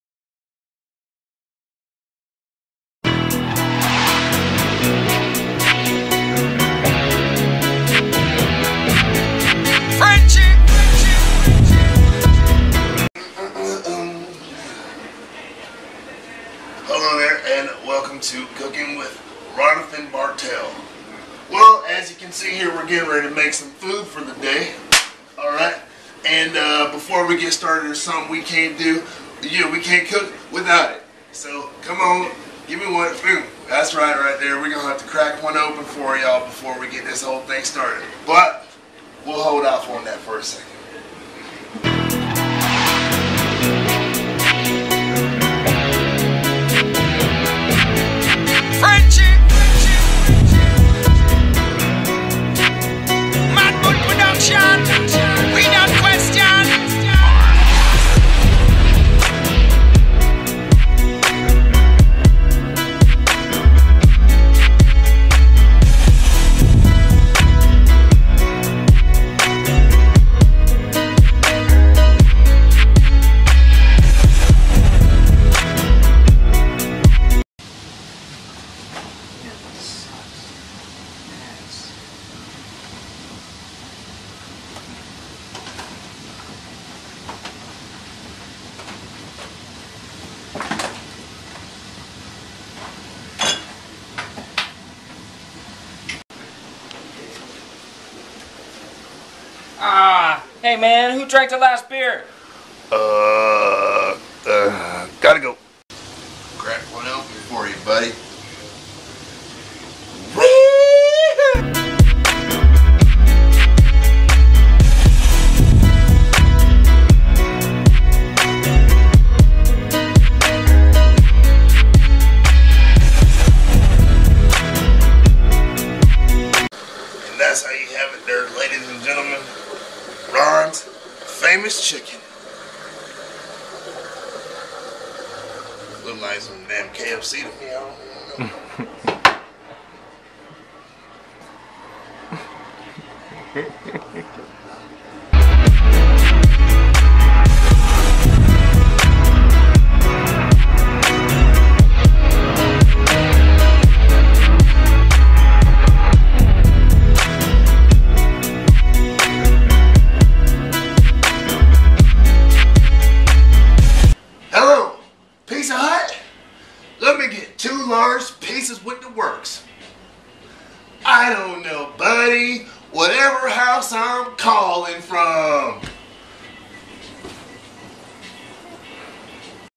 THE BRADLEY BROWN SHOW "COOKING WITH RONATHEN"
RONATHEN BARTELL SHOWS US HOW HE GETS DOWN IN THE KITCHEN